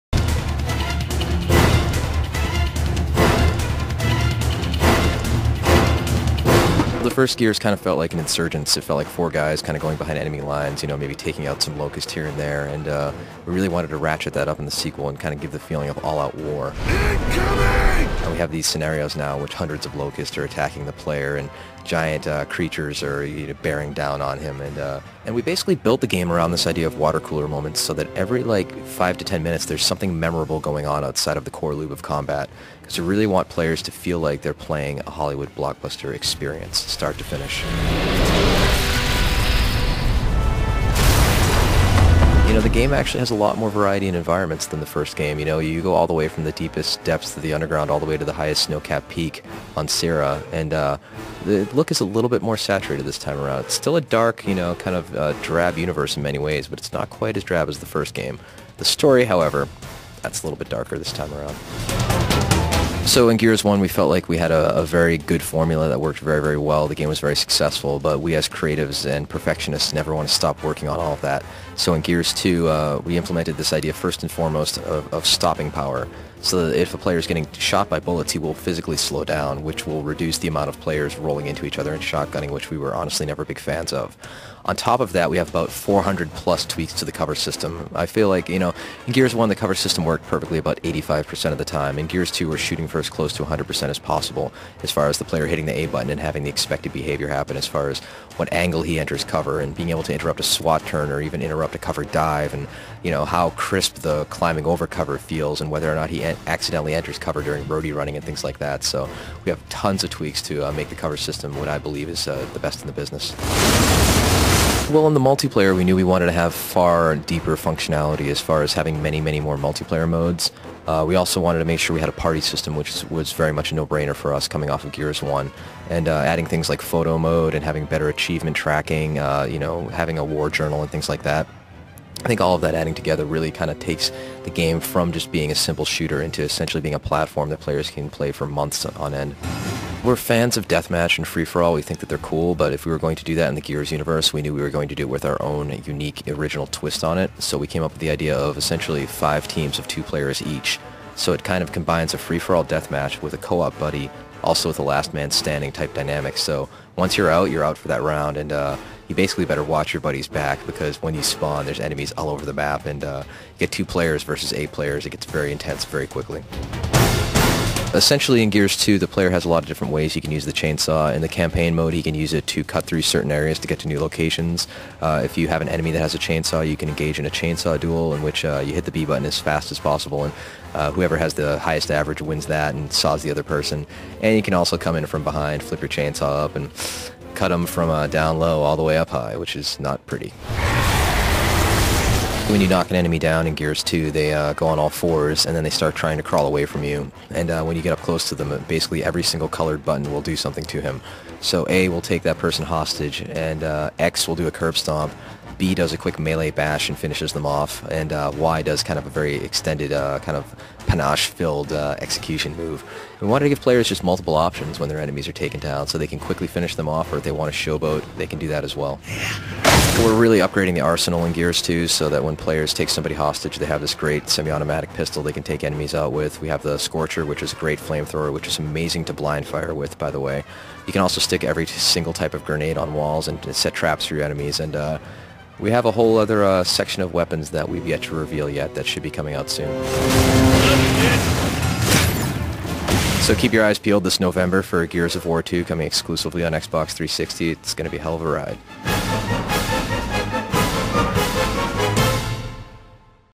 Gears of War 2 Cliff Bleszinski Interview
This is a video of GameTrailers.com interviewing Cliff Bleszinski about the sequel the one of the best games to ever hit the store shelves. He talks about new executions you can do in the game and a new game type: 5 Teams of 2.